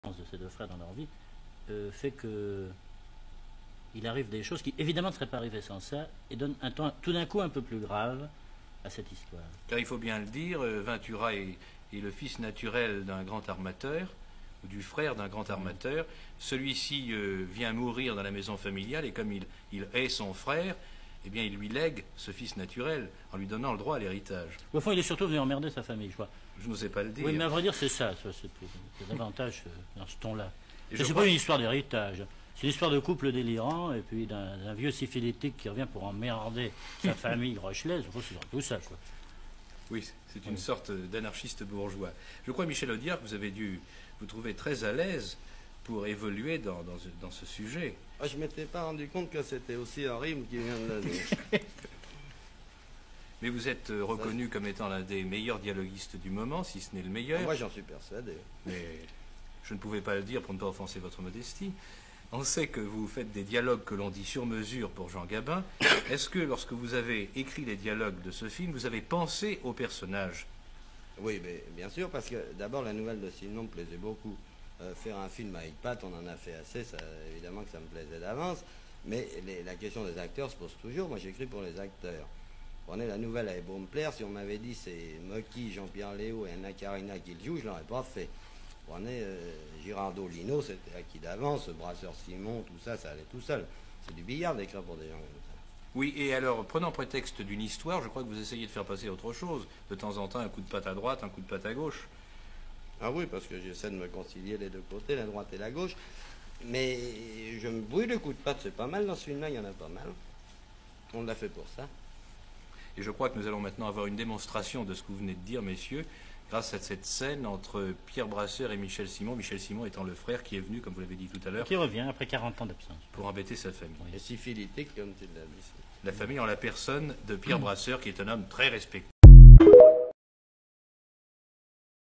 Partie 2/3 Interview de Denys de LA PATELLIERE (réalisateur) et de Michel AUDIARD (dialoguiste)
Interview de Denys de LA PATELLIERE (réalisateur) et de Michel AUDIARD (dialoguiste) par Mario BEUNAT à propos du film "Le bateau d'Emile", tiré d'une nouvelle de Georges SIMENON. Suivi de l'Interview des deux interprètes Annie GIRARDOT et Lino VENTURA.